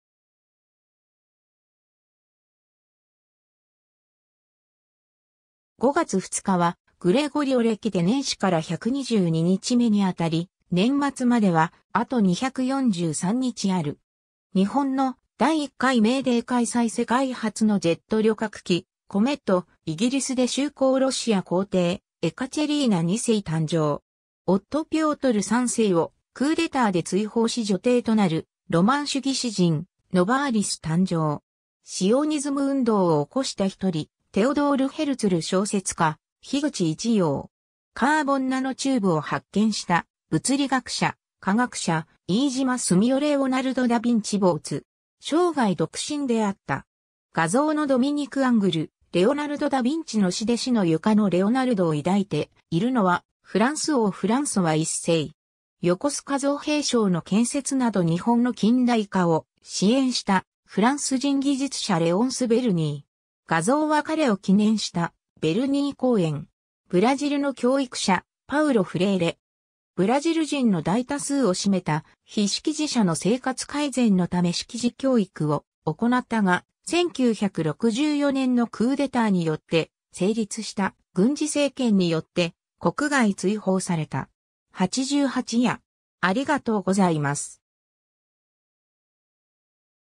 5月2日
5月2日（ごがつふつか）はグレゴリオ暦で年始から122日目（閏年では123日目）にあたり、年末まではあと243日ある。
 日本の第1回メーデー開催(1920) 世界初のジェット旅客機コメット、イギリスで就航(1952) ロシア皇帝、エカチェリーナ2世(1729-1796)誕生。
夫ピョートル3世をクーデターで追放し女帝となる ロマン主義詩人ノヴァーリス(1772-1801)誕生。
 シオニズム運動を起こした一人、テオドール・ヘルツル(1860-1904) 小説家、樋口一葉(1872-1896)。
 カーボンナノチューブを発見した物理学者・化学者、飯島澄男(1939-) レオナルド・ダ・ヴィンチ(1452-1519)没。
生涯独身であった。
画像のドミニク・アングル『レオナルド・ダ・ヴィンチの死』(1818)で死の床のレオナルドを抱いているのはフランス王フランソワ1世。
 横須賀造兵廠の建設など日本の近代化を支援したフランス人技術者レオンス・ヴェルニー(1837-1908)。
画像は彼を記念したヴェルニー公園。
 ブラジルの教育者、パウロ・フレイレ(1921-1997)。
ブラジル人の大多数を占めた非識字者の生活改善のため識字教育を行ったが、1964年のクーデターによって成立した軍事政権によって国外追放された。
 八十八夜（2日頃）